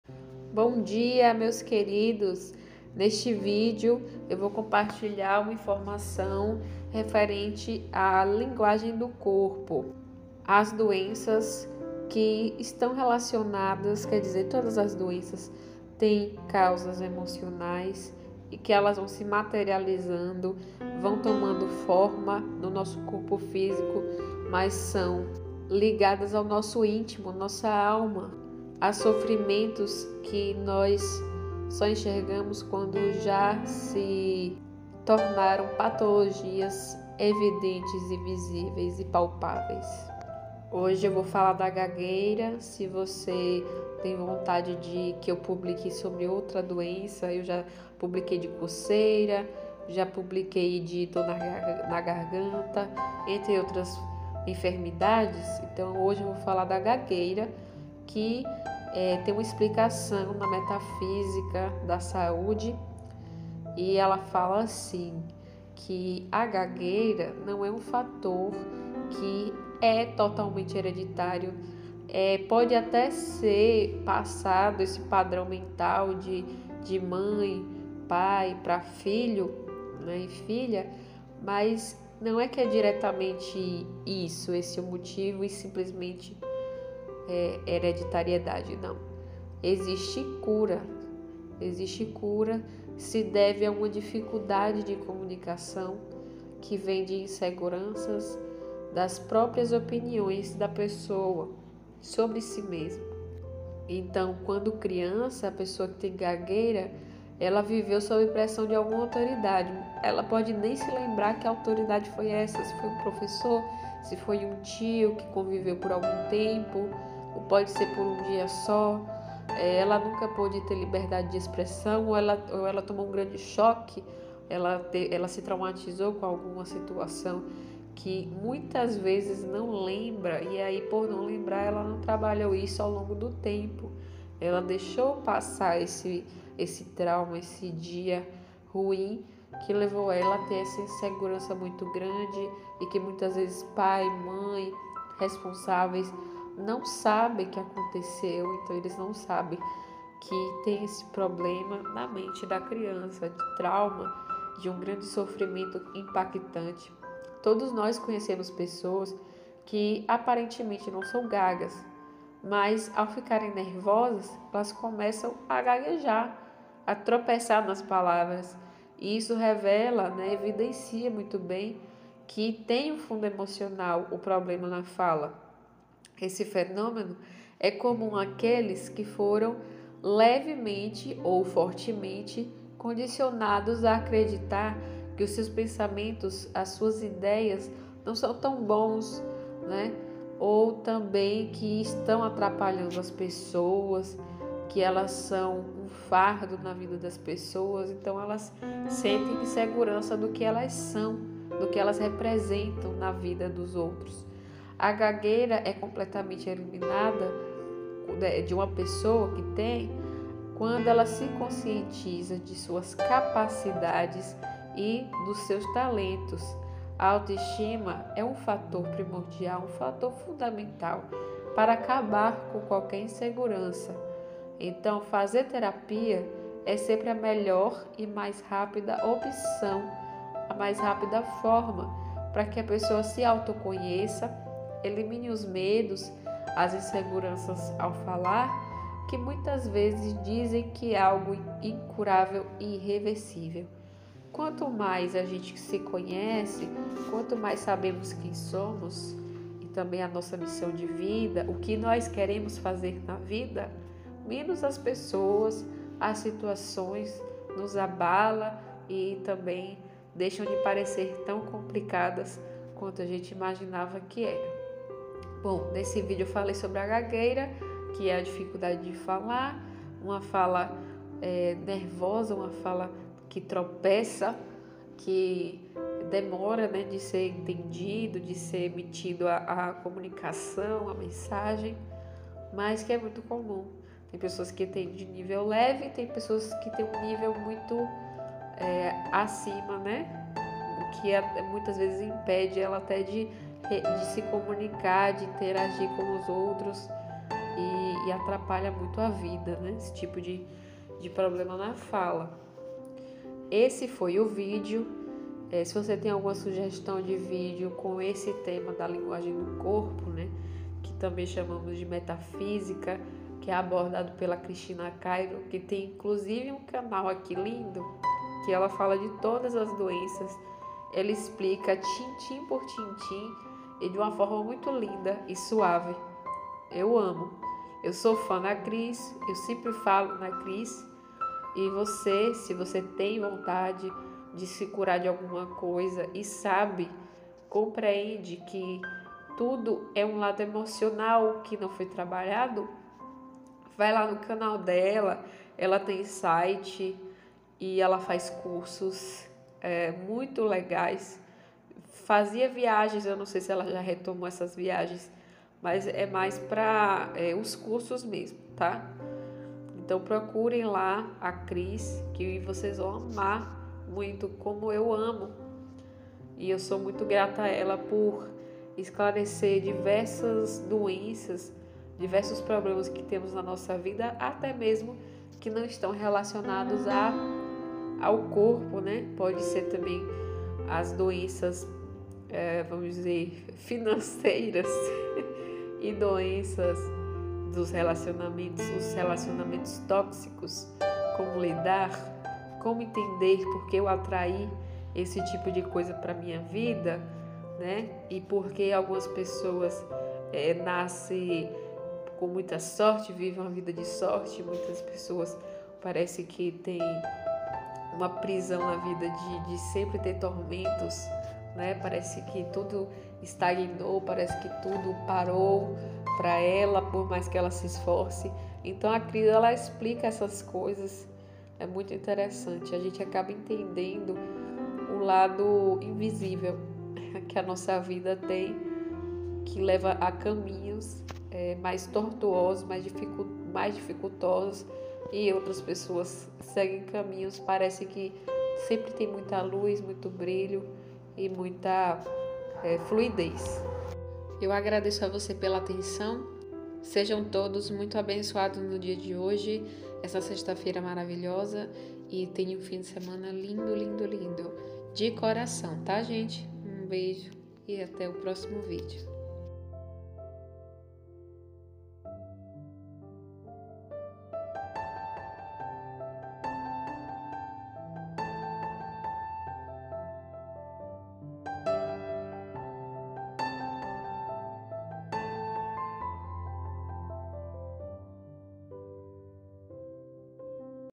A GAGUEIRA SEGUNDO A LINGUAGEM DO CORPO E METAFÍSICA DA SAÚDE
Vídeo sobre os problemas relacionados a fala.